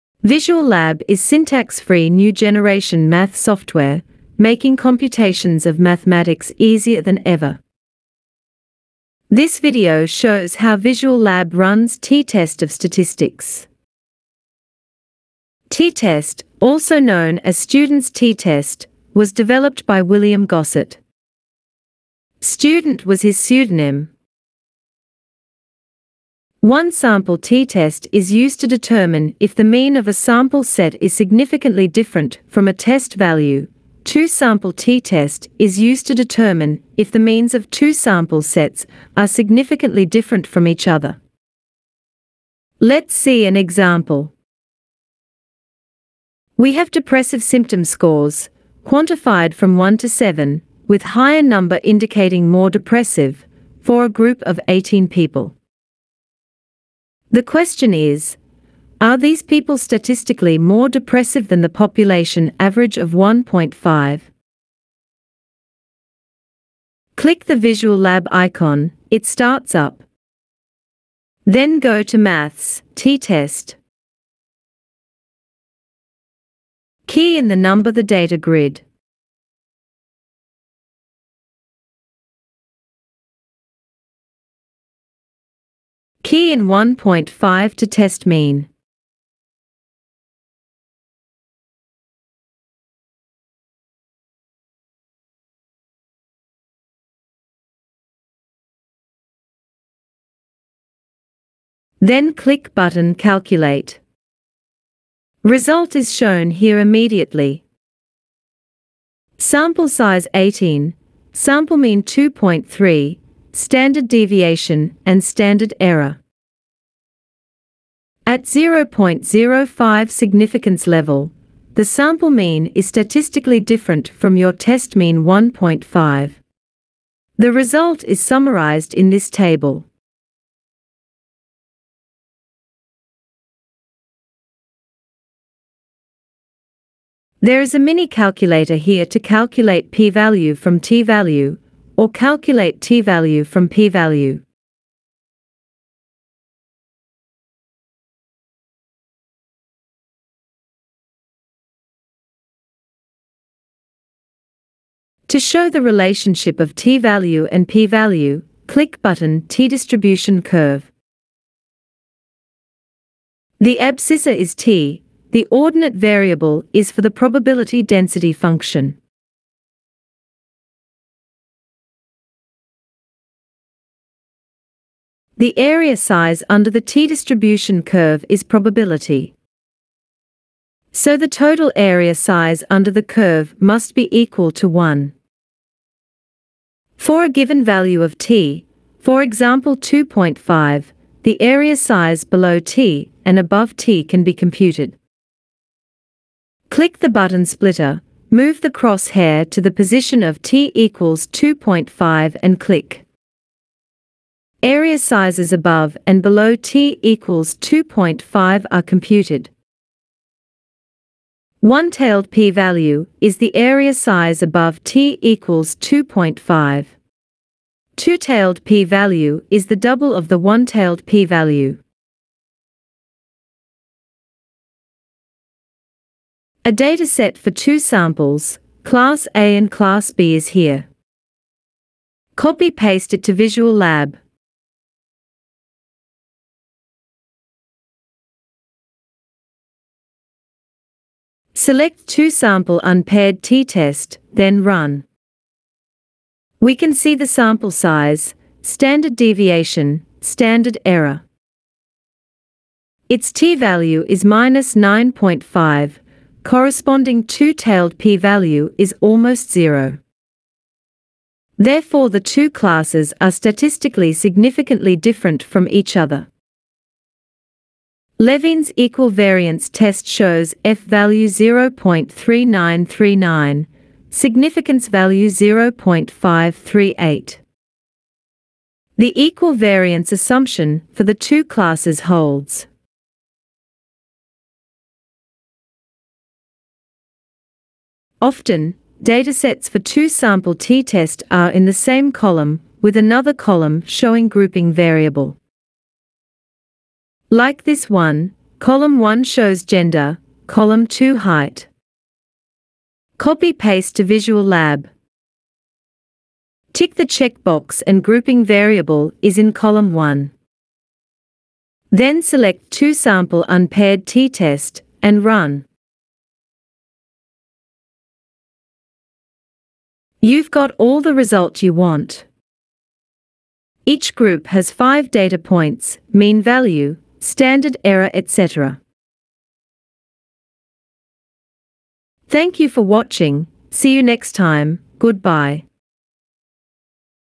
VisualLab runs t-test statistics Student's t-test
This video shows some examples how VisualLab run Student’s t-test in statistics, including both one sample t-test and two sample t-test.